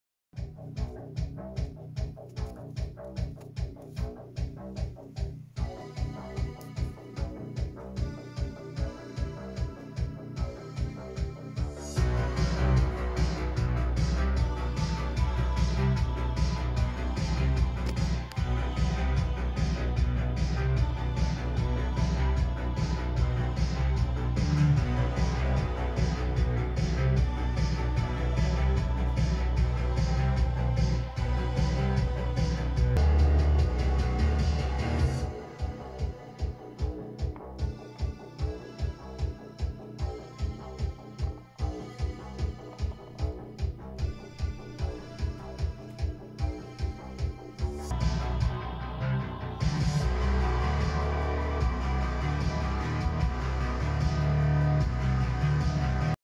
VHS Philips VR231
Philips VR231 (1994) [Chassis: Michelle] - Einfacher "TurboDrive"-Videorekorder, eines der früheren Modelle.
Philips VR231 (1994) [Chassis: Michelle] - basic "TurboDrive"-VCR, early model.